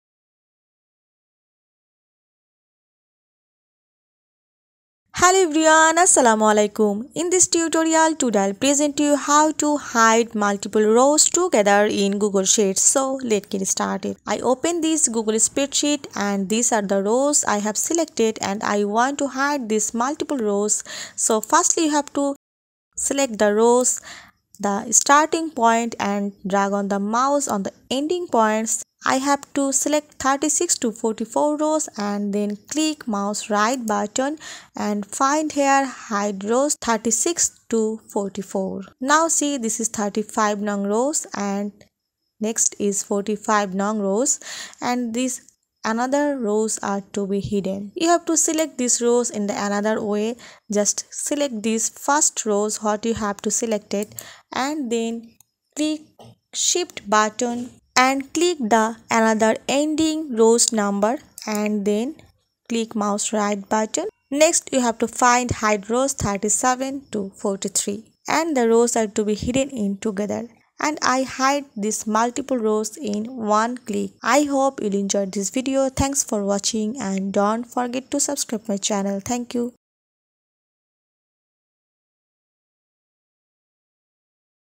How to hide multiple rows together in Google Sheets
Hello everyone, Today I will present you, How to hide multiple rows together in Google Sheets.